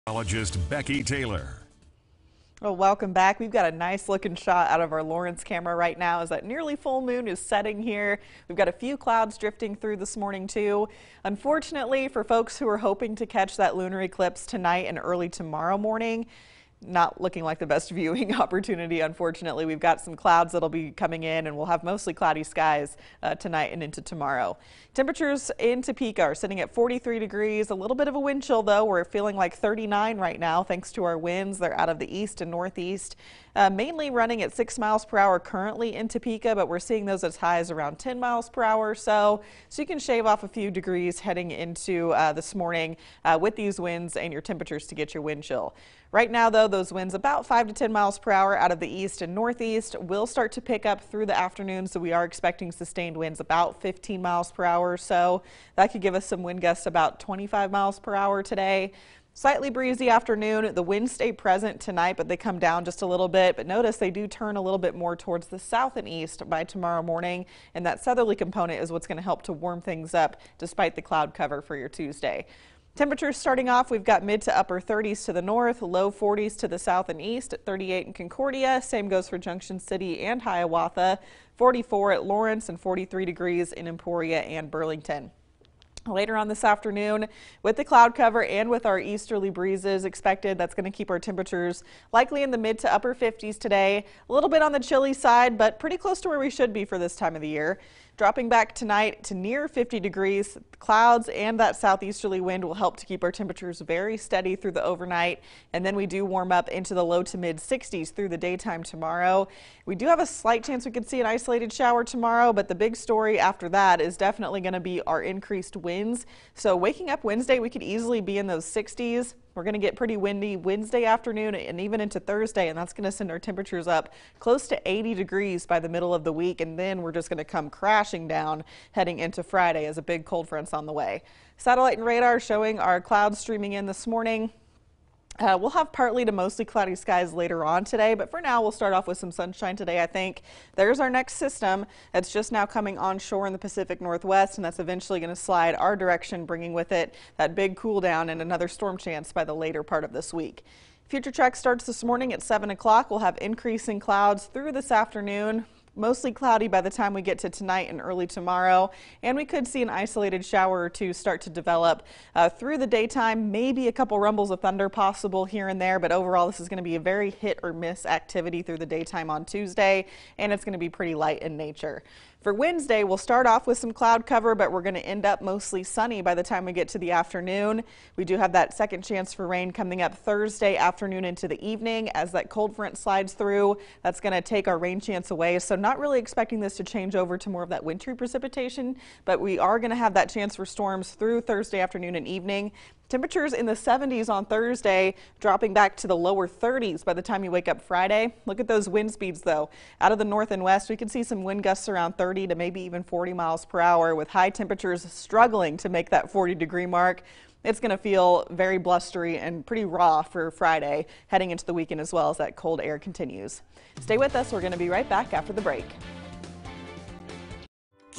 Latest Forecast Update
Mild start to the week ahead of a big temperature swing